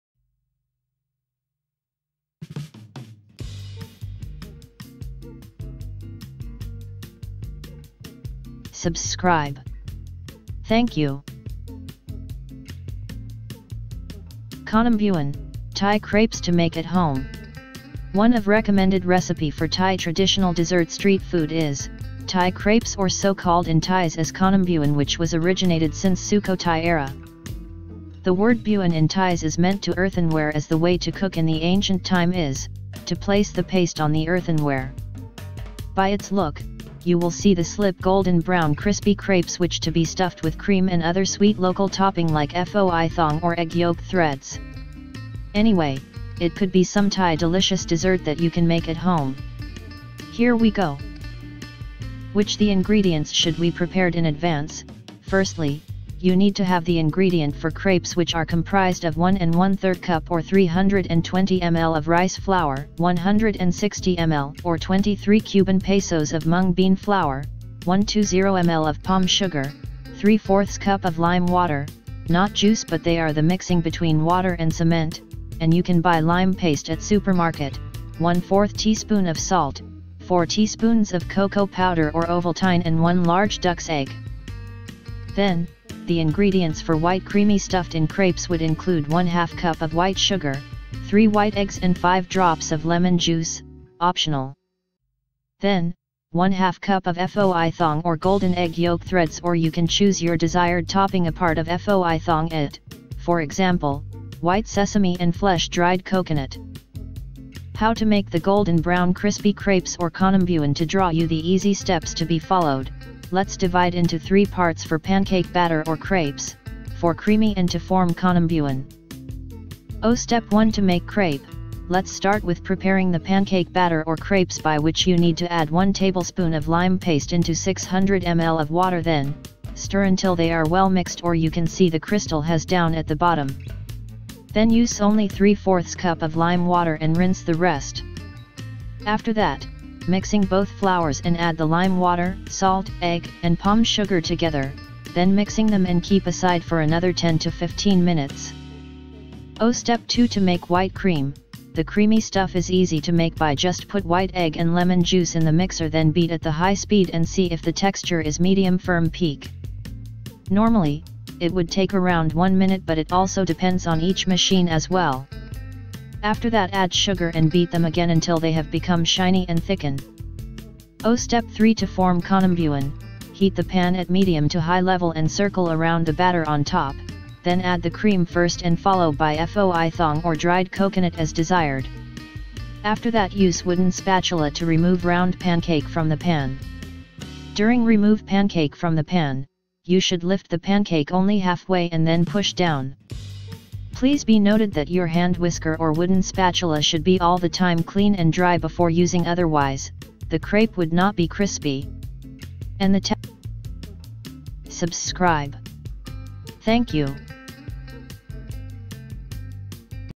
KhanomBuang, Thai Crepes to Make At Home [PICTURE]
One of recommended recipe for Thai Traditional dessert street food is, Thai Crepes or so-called in Thais as KhanomBuang which was originated since Sukhothai era. The word ‘Buang’ in Thais is meant to earthenware as the way to cook in the ancient time is, to place the paste on the earthenware. By its look, you will see the slip golden brown crispy crepes which to be stuffed with cream and other sweet local topping like Foi Thong or egg yolk threads. Anyway, it could be some Thai delicious dessert that you can make at home. Here we go.